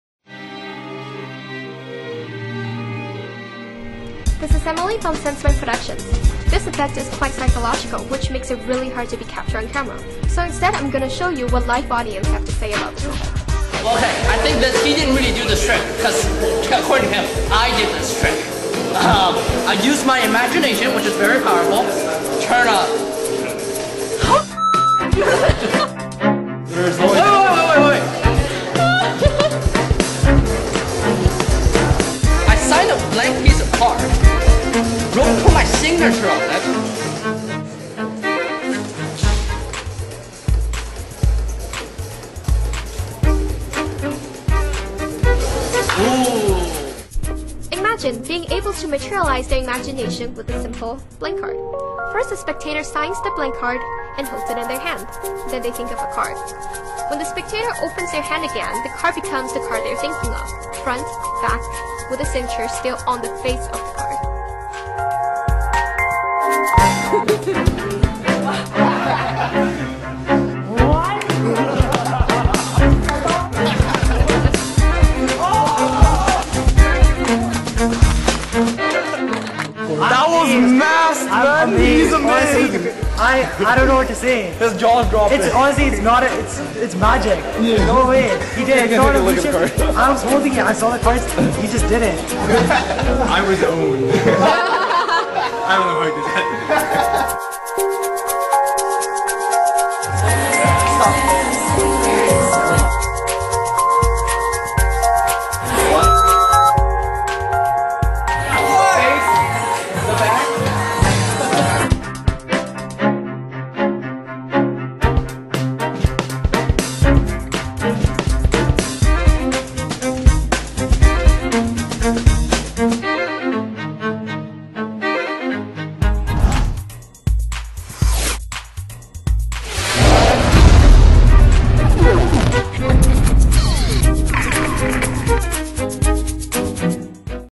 Imagine - G et SansMinds
Une carte blanche est signée par un spectateur. Puis une carte est choisie dans un jeu. L'image de la carte choisie vient alors s'imprimer mystérieusement sur la carte blanche signée que le spectateur tenait pourtant entre ses mains !